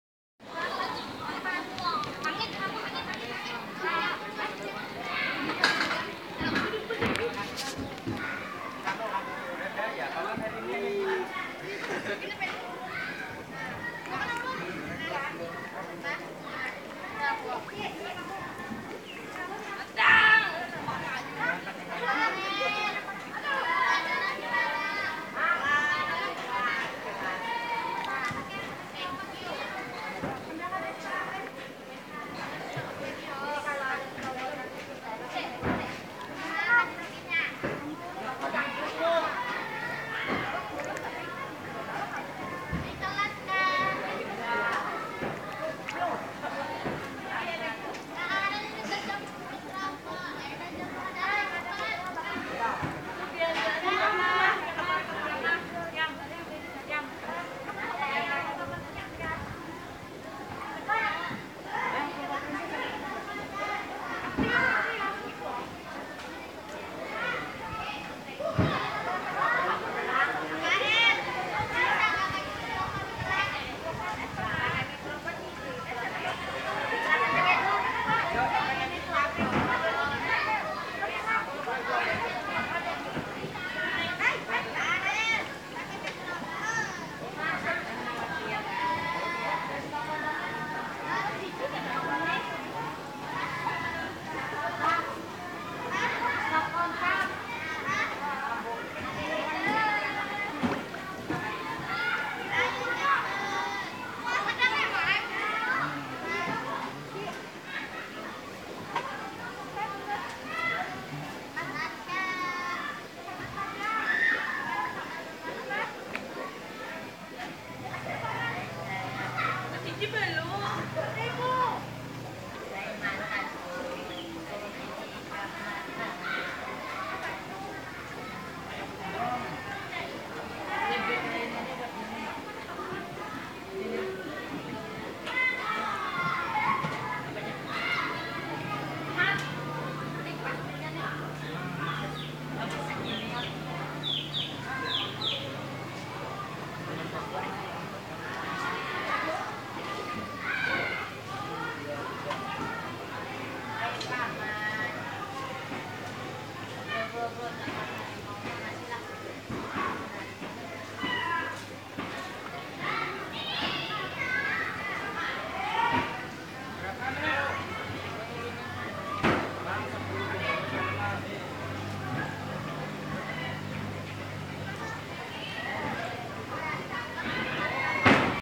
Lele jumbo usia enam minggu di bioflok Ponpes Al-Amin Kapuas
INFORMASI KAPUAS - Direkam pada hari Rabu, 21 November 2018.